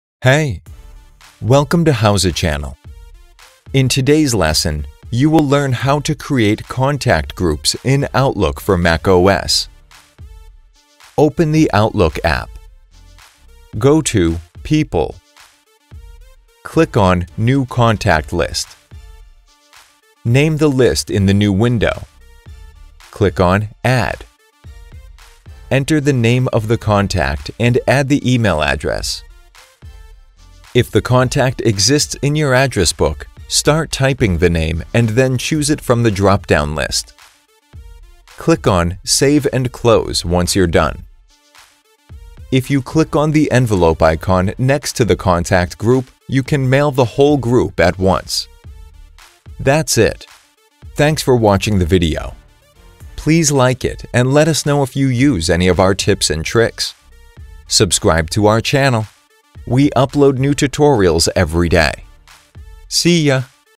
How to Create Contact Groups in Outlook for Mac | Microsoft Office for macOS
In today's lesson, you will learn how to create contact groups in Outlook for macOS.
Open Outlook app. Go to People. Click on ‘New Contact List’. Name the list in the new window. Click on Add.
If the contact exists in your address book, start typing the name and then choose it from the dropdown list. Click on Save & Close once you’re done.
If you click on the envelope icon next to the contact group, you can mail to the whole group at once.